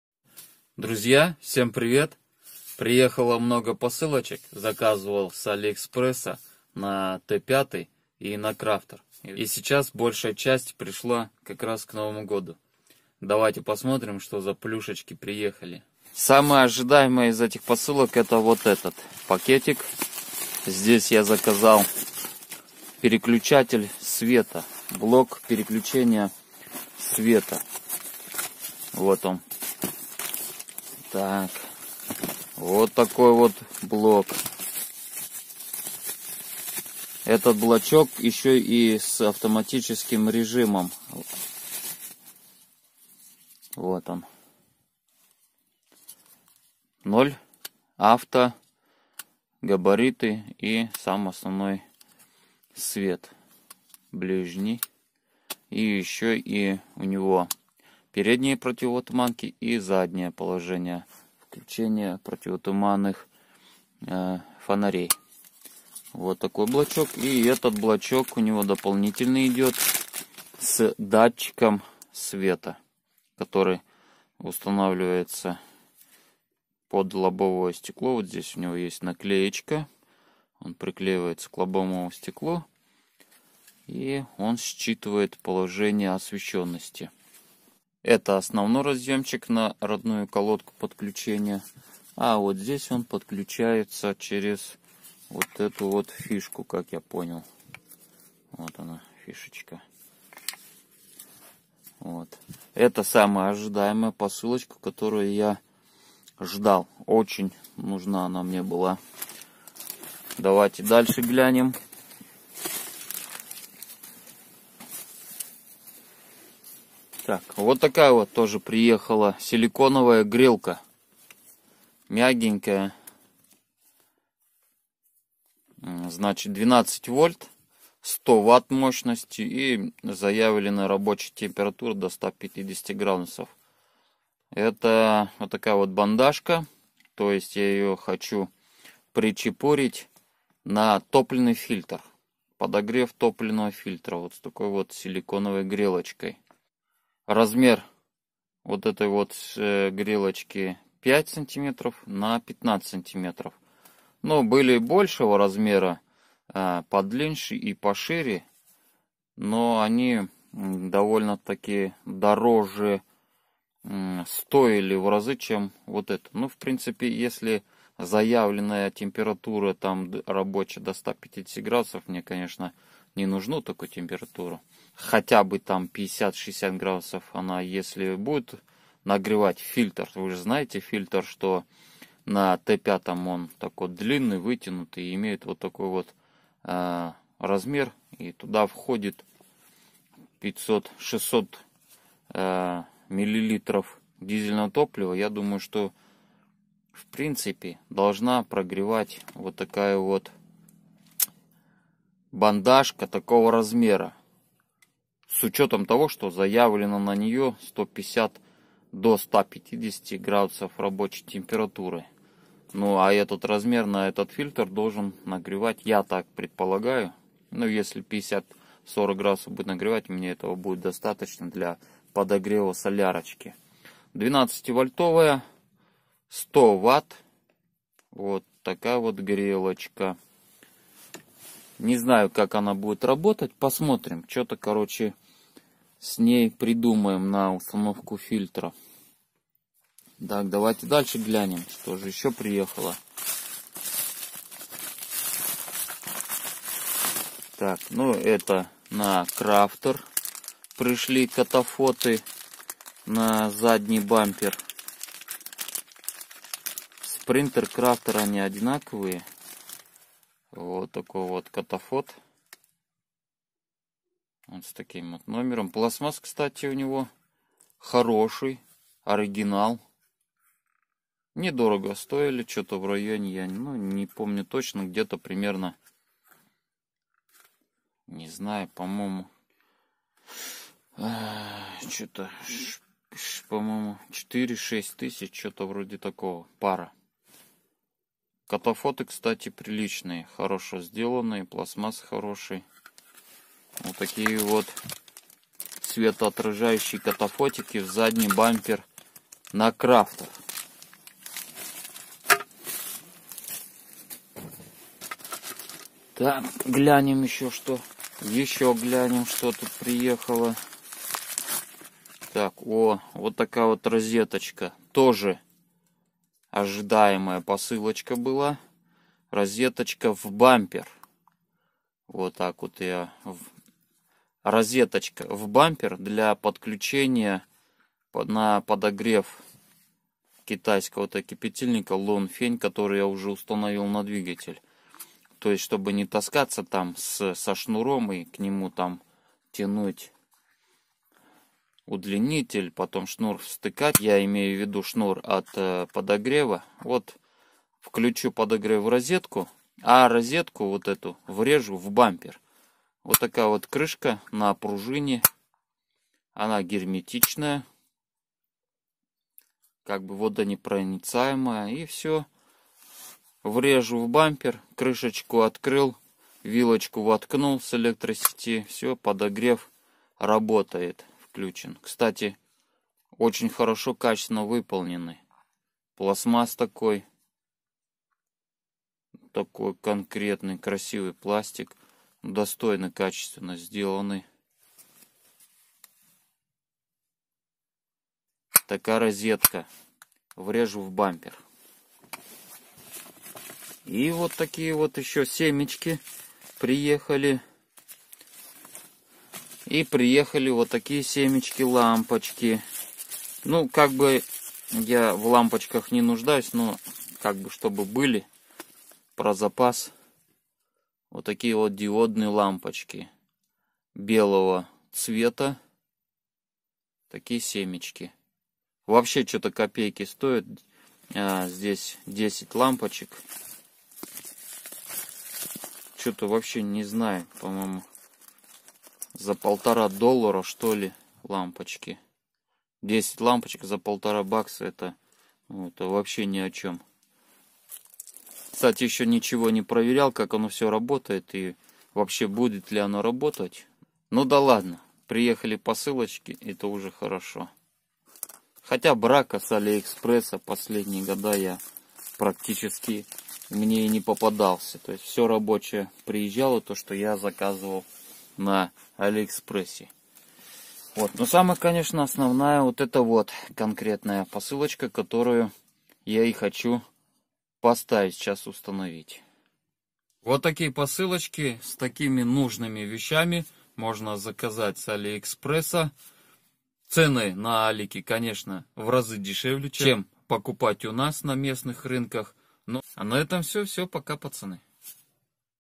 Распаковка ТОПОВЫХ АВТО ТОВАРОВ с АЛИЭКСПРЕСС ®️
КПД 78. Обзор посылок.  Заказал Блок Переключатель света фар с авто включением,  Силиконовую нагревательную грелку для подогрева фильтра, Фонари катафоты для фольксваген Крафтер - мерседес спринтер ®️